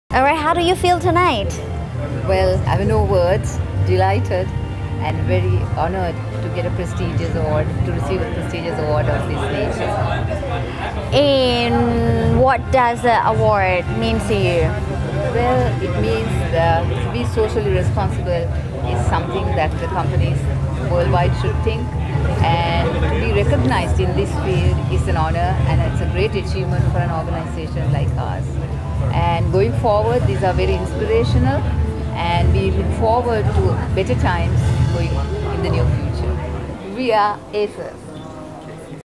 Hemas Holdings PLC - Asia's Most Socially Responsible Company Of The Year - ACES Awards 2019
ACES Awards - Showcasing Asia's Finest Responsible Corporate Leaders since 2014.

The Asia Corporate Excellence & Sustainability Awards (ACES) is one of the most prestigious accolades that recognises inspiring leaders and sustainability advocates cross-industry and across Asia.